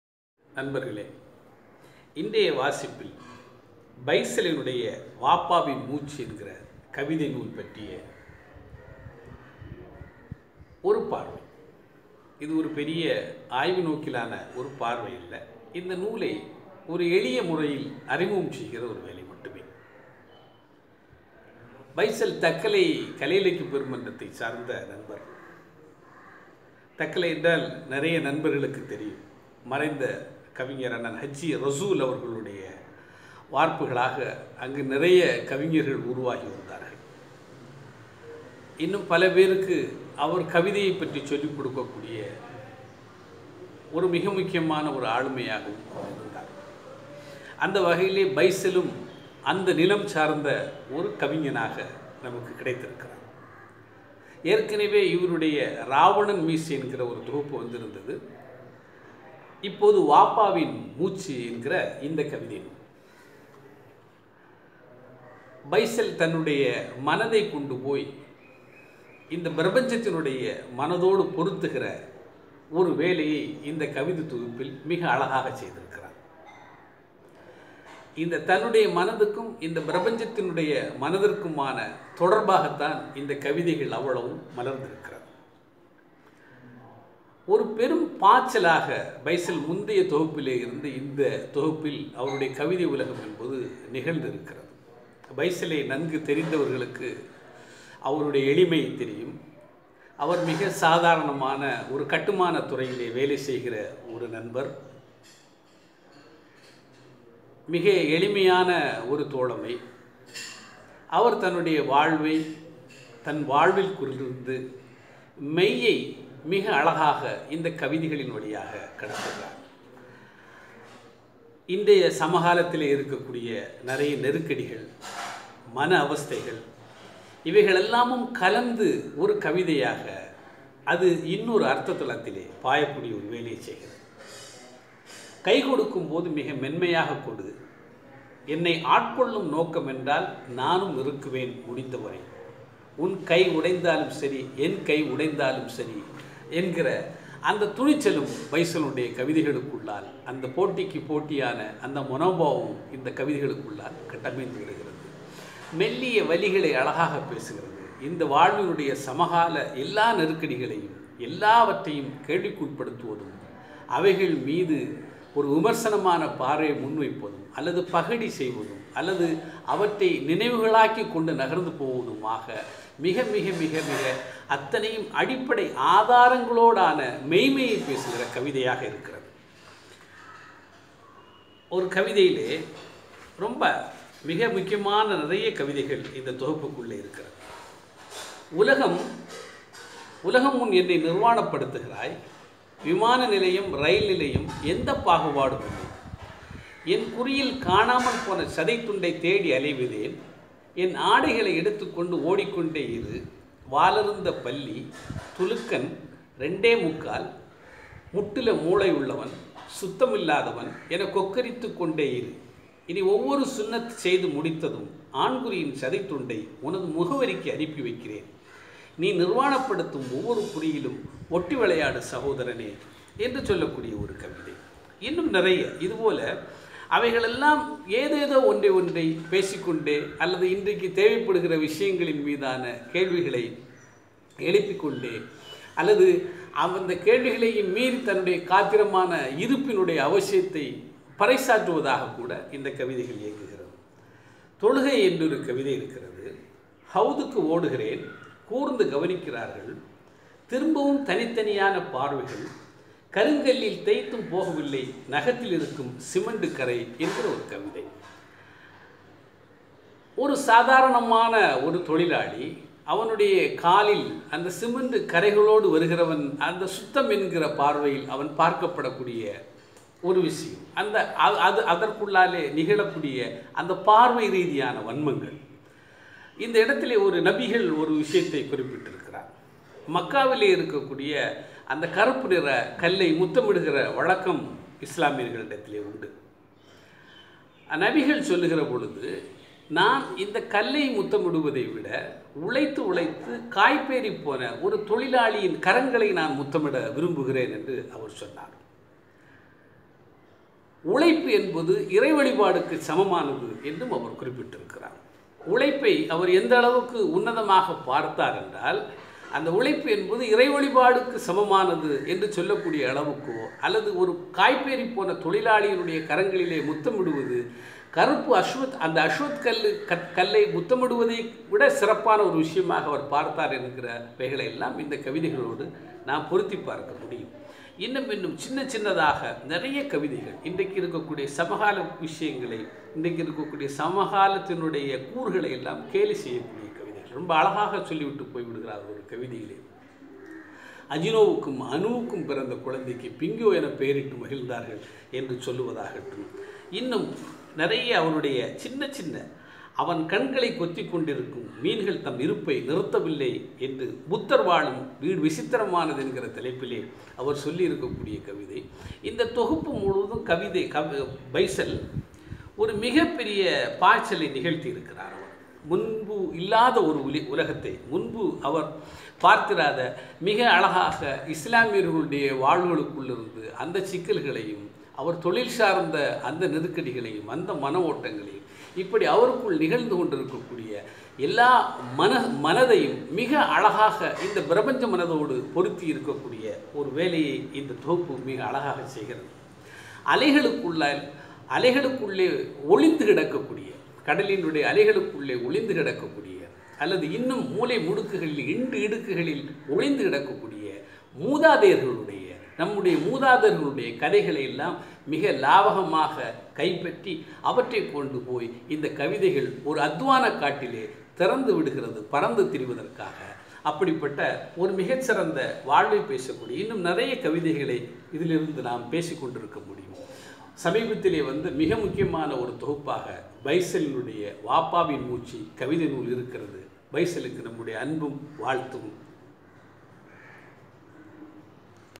பைசலின்"வாப்பாவின் மூச்சு"கவிதை நூல்
நூல் அறிமுக உரை
எழுத்தாளர்.மீரான் மைதீன்.